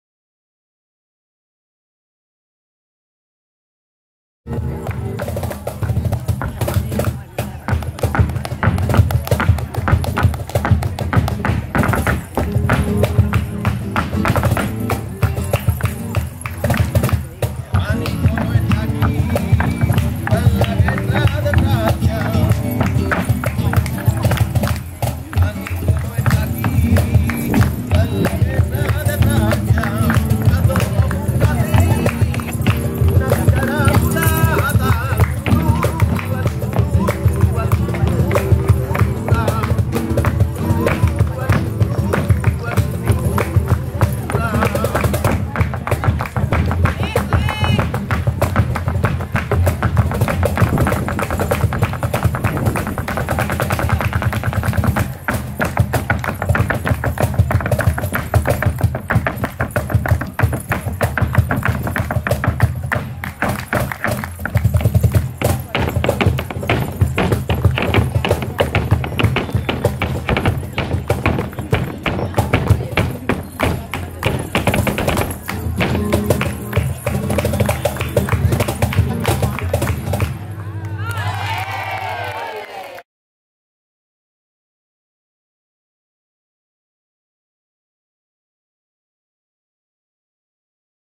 Bryanna Kika Masala - Flamenco Dance - FLARF 2016
Bryanna Kika Masala of the Masala Gypsy Dancers performing at the Florida Renaissance Festival - 3/19/2016